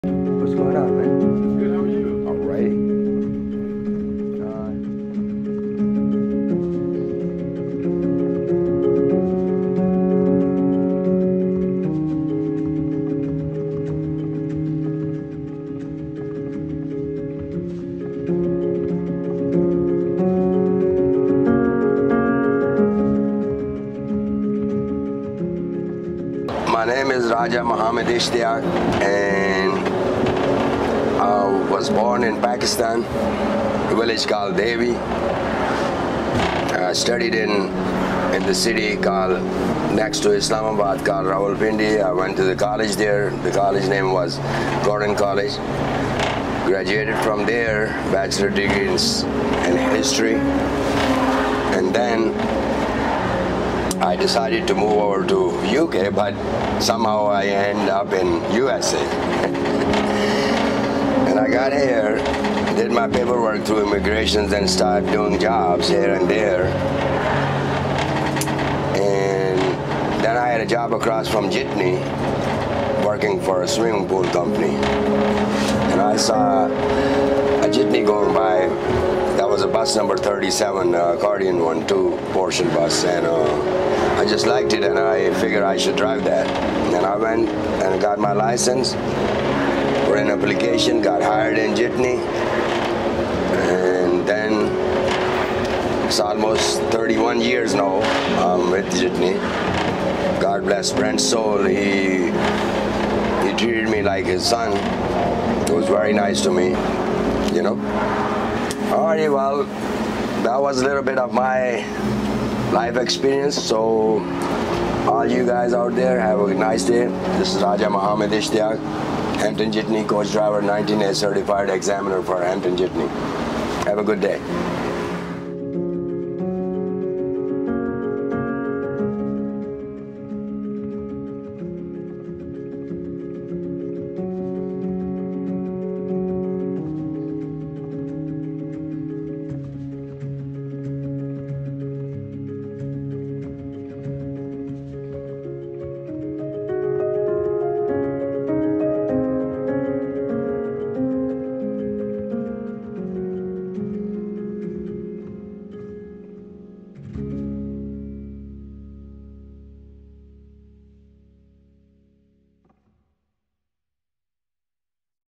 Meet ABA's Motorcoach Driver of the Year: Raj Ishtiaq from Hampton Jitney.
Meet Raj Ishtiaq a driver from Hampton Jitney Transportation in Calverton, N.Y.